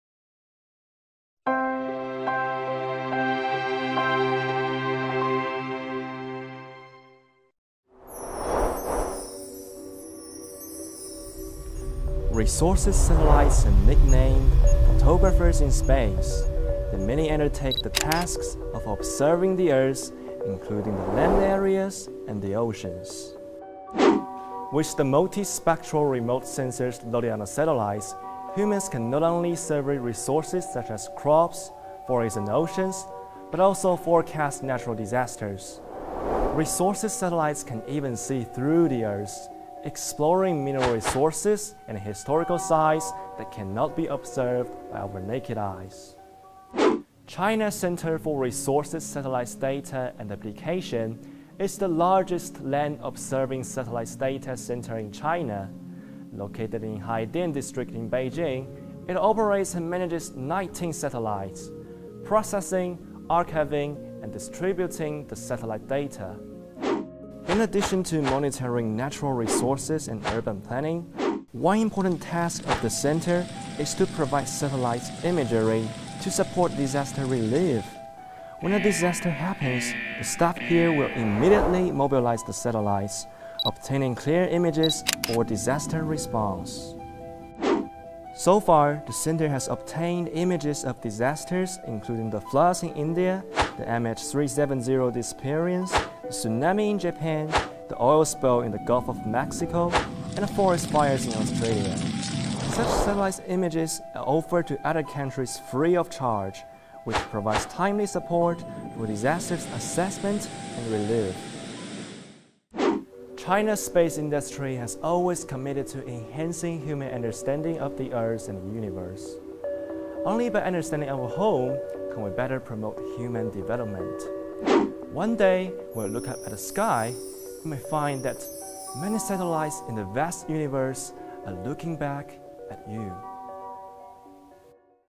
The Eyes of Space
Navigation, TV broadcast, weather forecast … Modern human living is dependent on satellites. Satellites undertake the important task of observing the earth. China Resources Satellite Application Center operates and manages 19 satellites, which not only monitor natural resources and urban planning, but also provides satellite image support for major disaster relief aid. Watch this video to see how "Space Eye" observes the Earth.